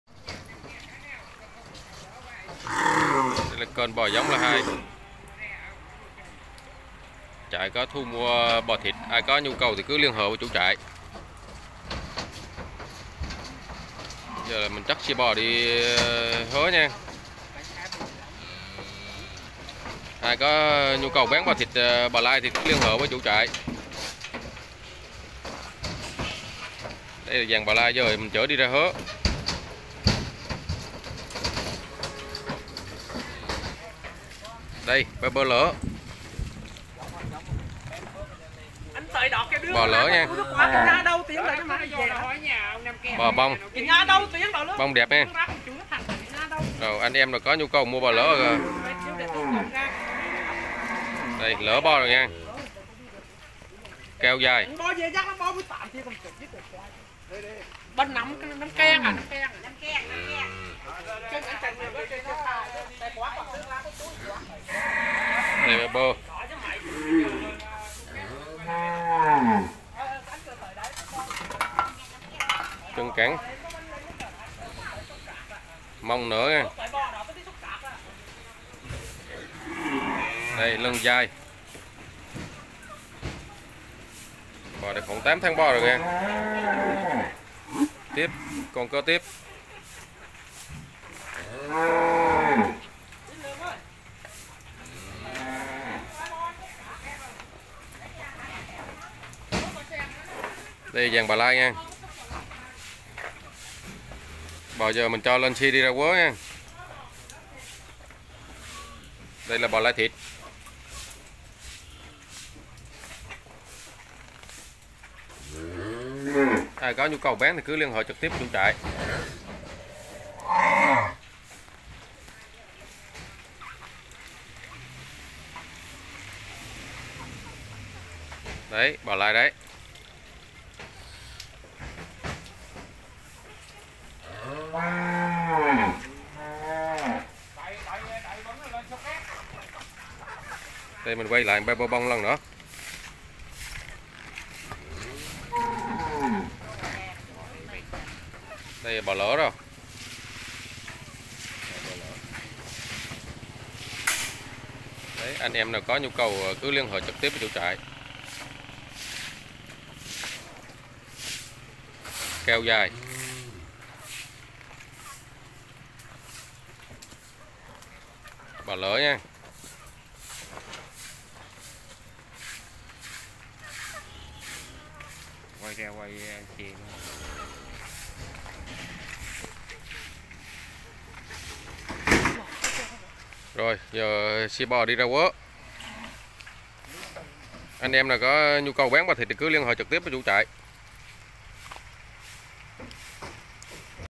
bò giống la hai 0386308995 bò 3b bông 3b vỗ béo, bò thịt
hôm nay trại bò giống la hai báo giá bò thịt, 3b vỗ béo, BÒ GIỐNG LA HAI